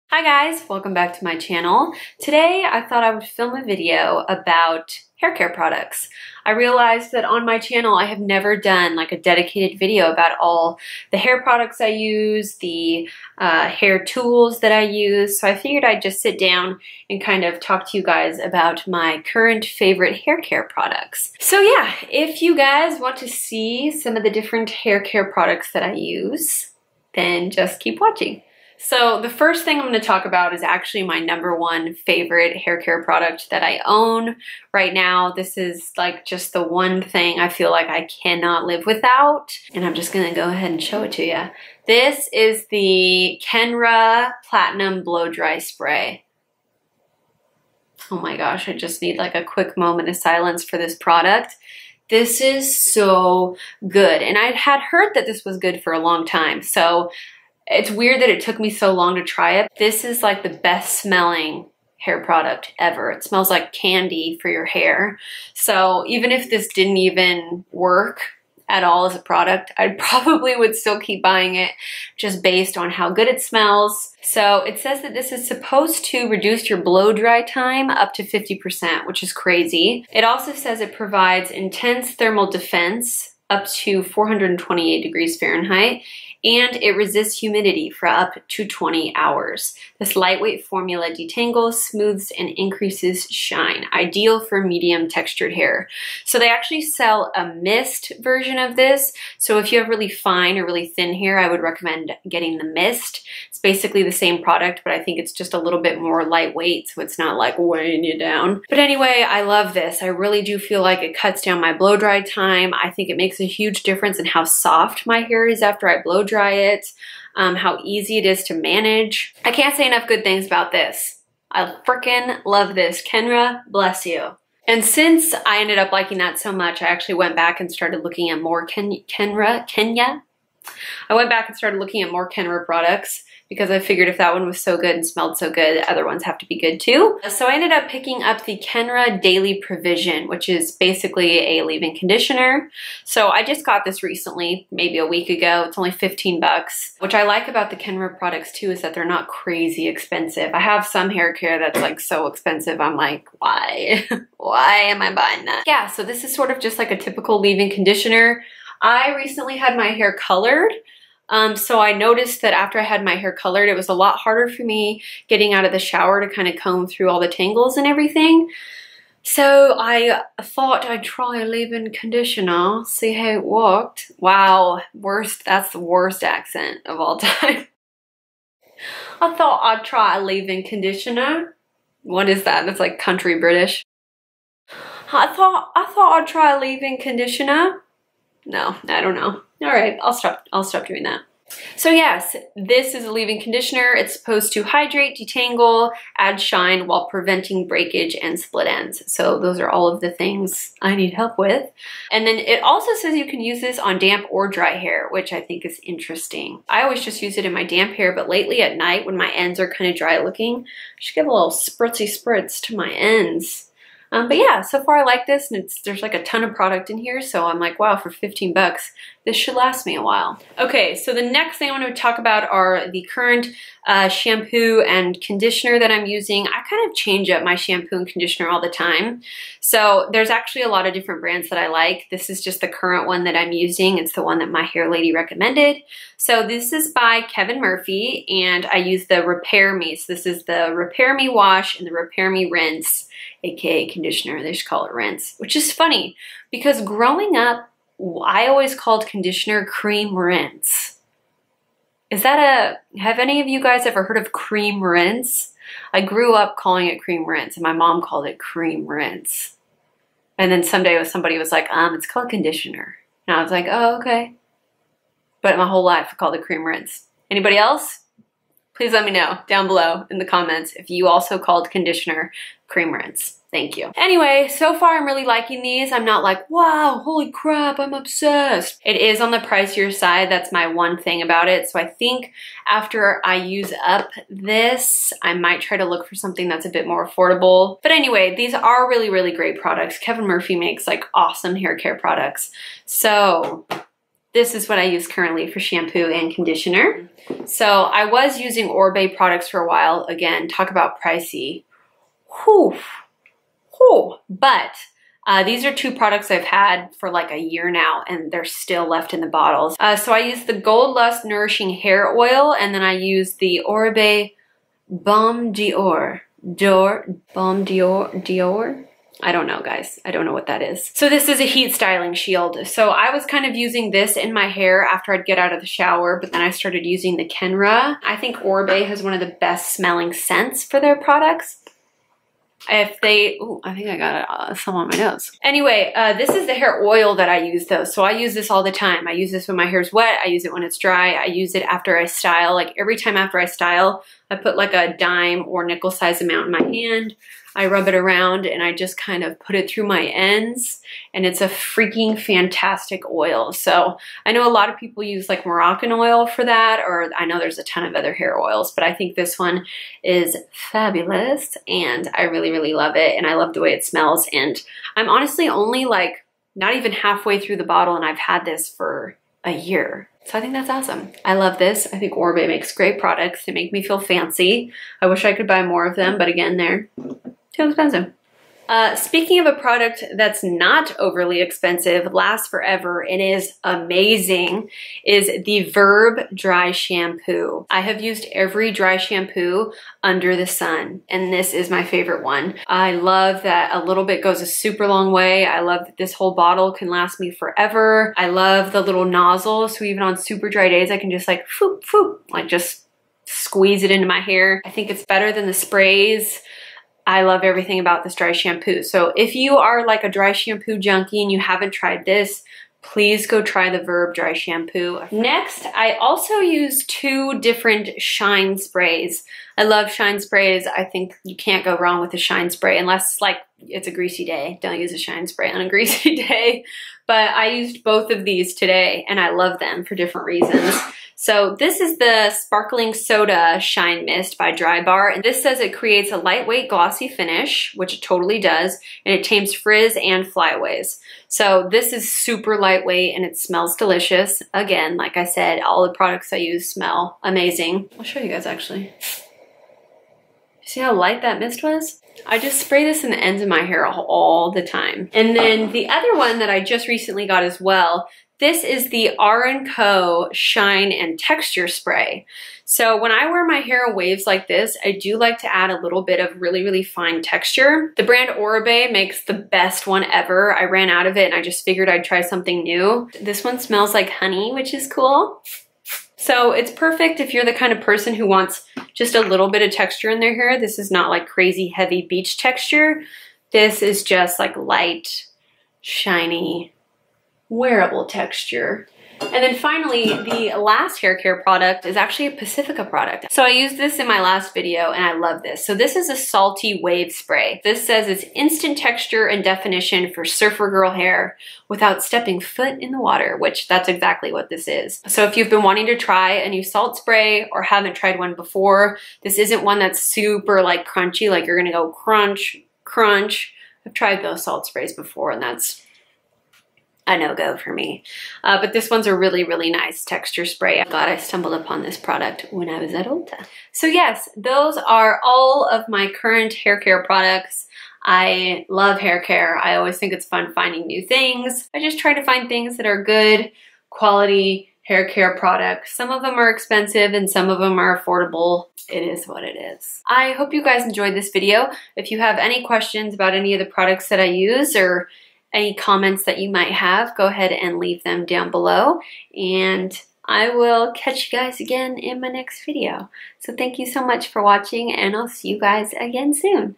My Favorite Hair Care Products!
Hi everybody! This video is straight up all about the products I use in my hair. I've never done a dedicated haircare video, so here it is! All products I purchased myself.

Products featured in this video:
Kenra Platinum Blow Dry Spray
Kenra Daily Provision
Kevin Murphy Repair Shampoo & Rinse
Oribe Gold Lust Hair Oil
Oribe Balm D'Or
Verb Dry Shampoo
Drybar Sparkling Soda Shine Mist
R+Co Trophy Shine & Texture Spray
Pacifica Salt Wave Spray